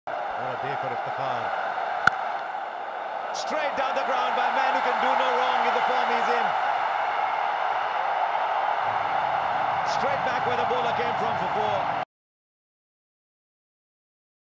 ICC Men's T20 World Cup 2022 | Greatest Rivalry's 2nd Innings Powerplay | INDvPAK
The stage is all set for a nail-biting chase in the GreatestRivalry! 🤩

Do not miss out on the LIVE action from ICC Men's T20WorldCup. Watch now, only on Star Sports & Disney+Hotstar!